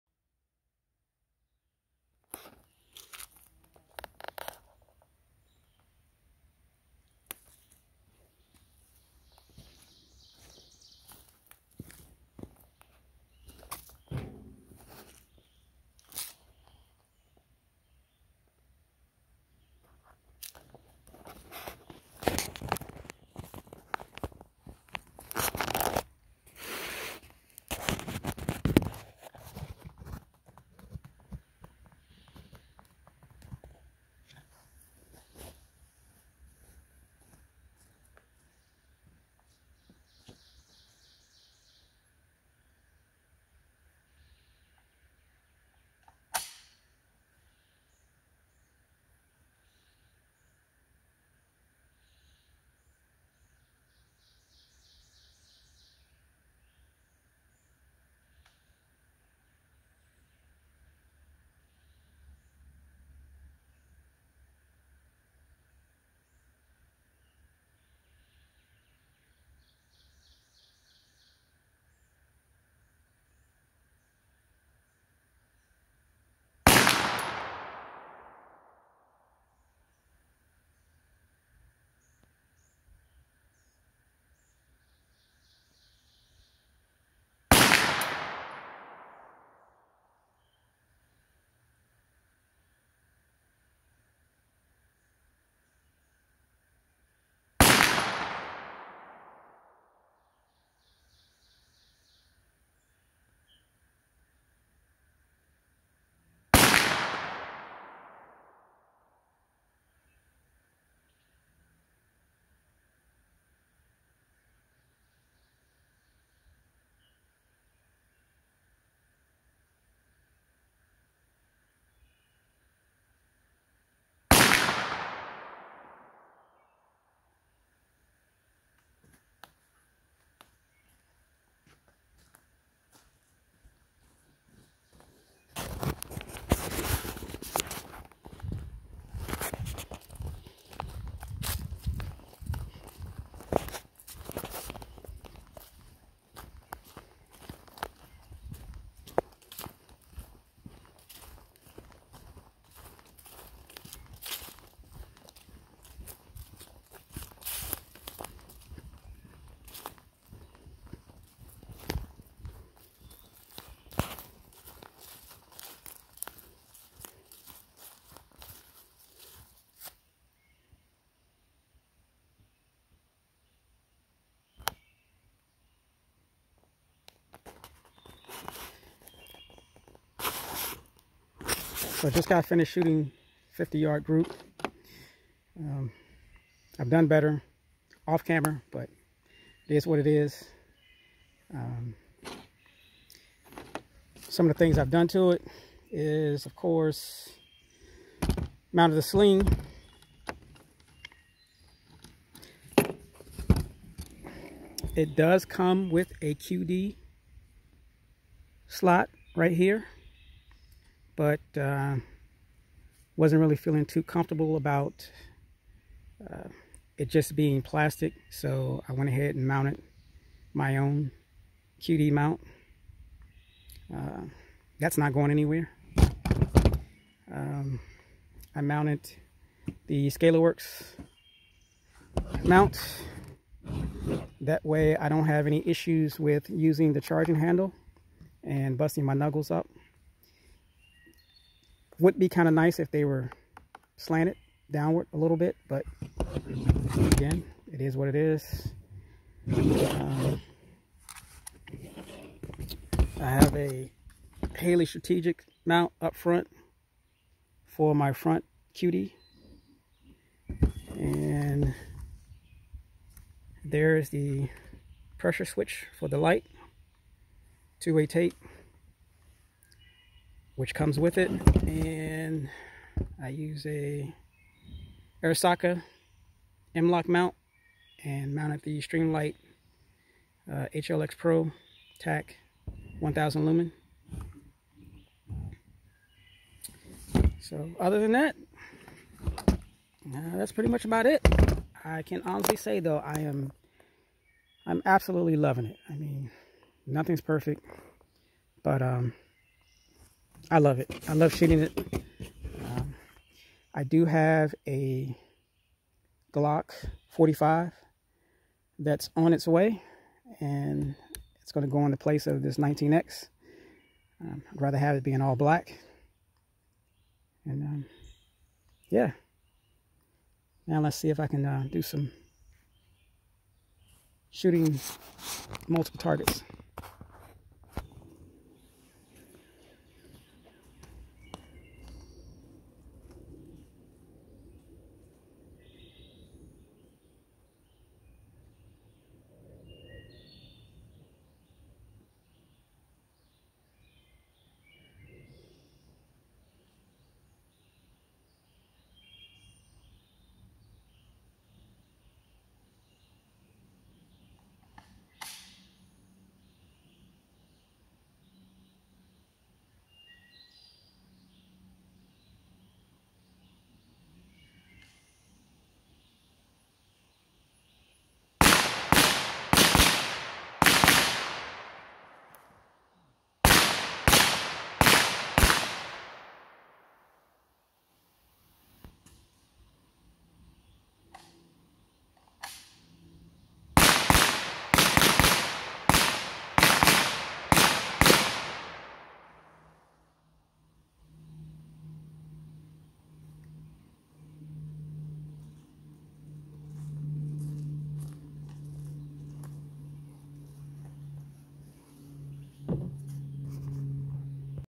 Meta Tactical 50 yards and quick observation
Meta Tactical Glock conversion 50 yard group plus a few additions i added to the rifle.  I've  actually had better groups at 100 yards.   I will do anther accuracy test at 100 yards once i have my Glock 45.